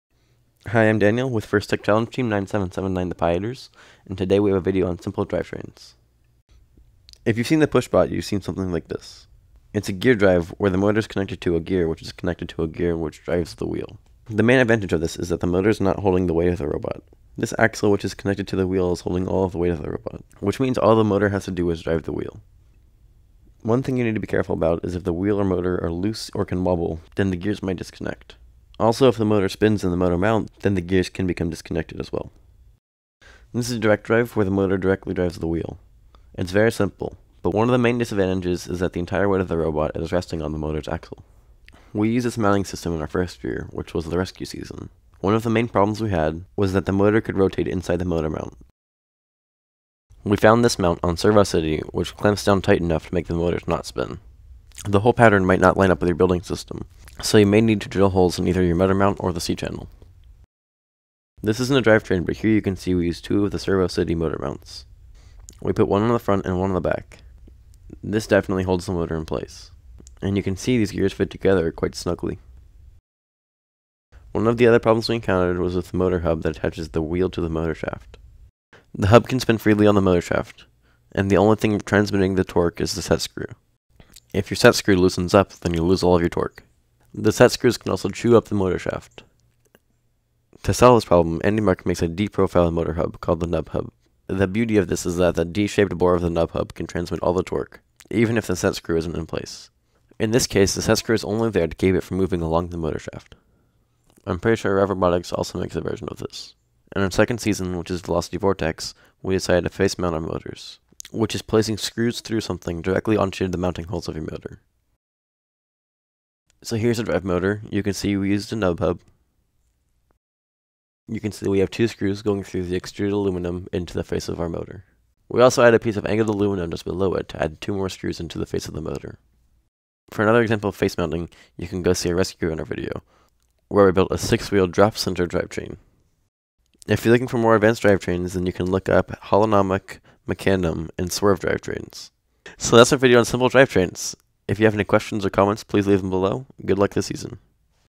FTC Robotics Help - Super Simple Beginner Drive Train Basics
We discuss direct drive and gear drive and some of the problems that often come, like gears not engaging and motors spinning in the motor mounts.